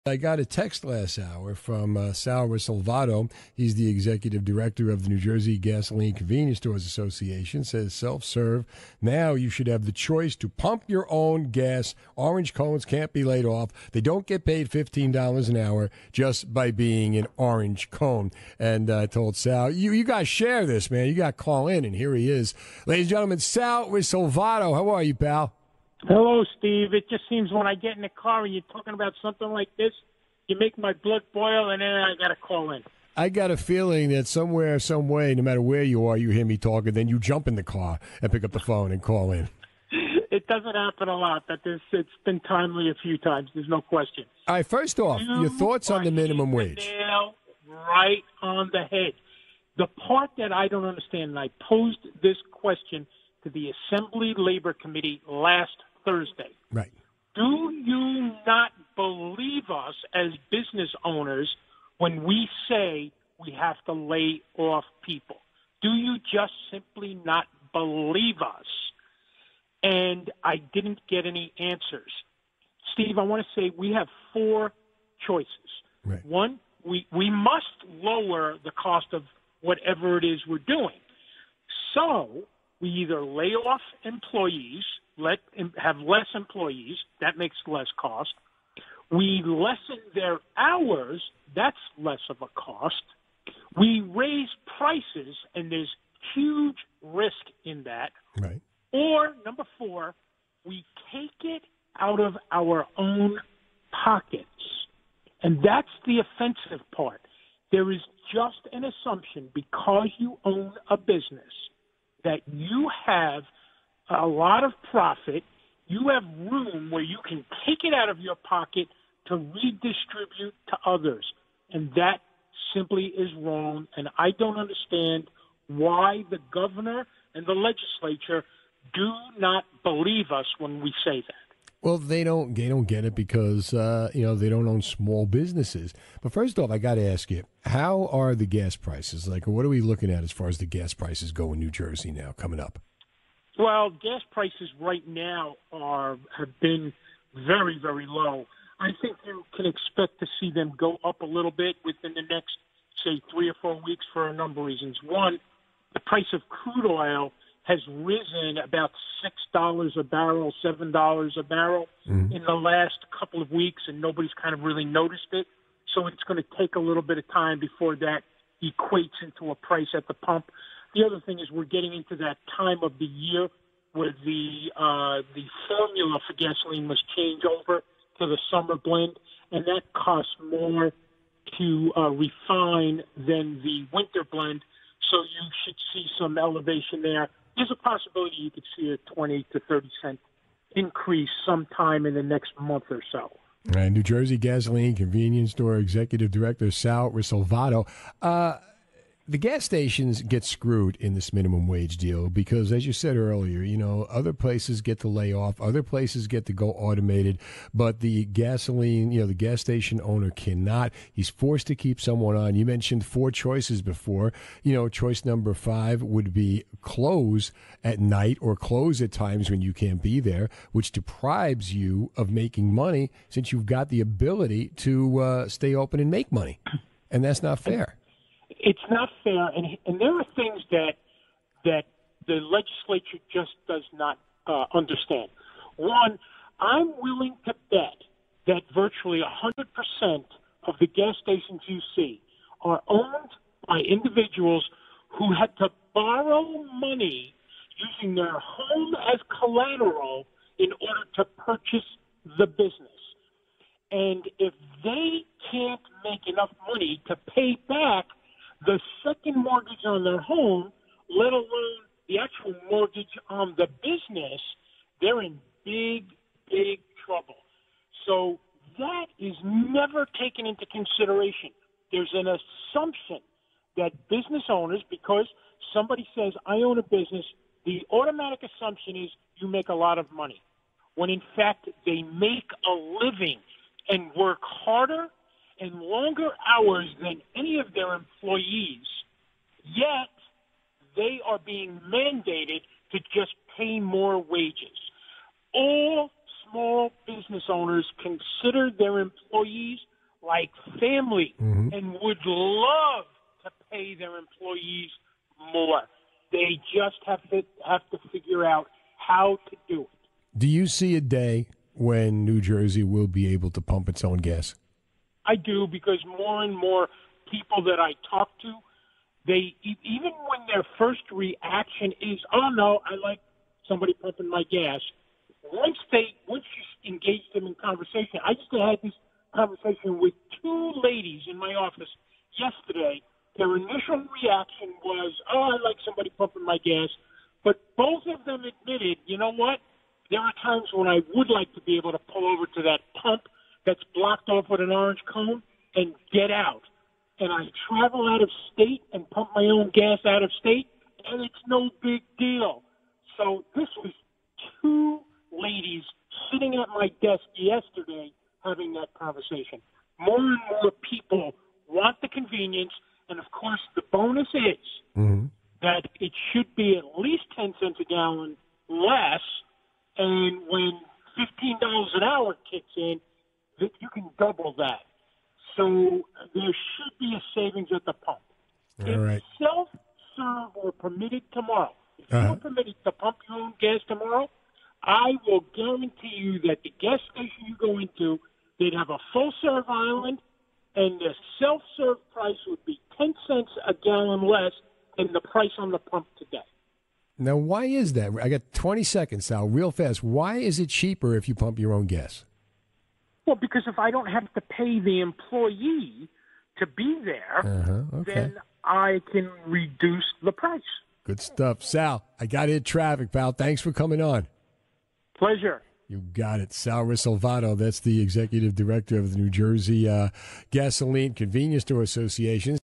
Sal Risalvato talks to Trev about pumping our own gas
Sal Risalvato, Executive Director of the New Jersey Gasoline, Convenience Store and Automotive Association agrees with Steve Trevelise that it's time to pump our own gas in NJ.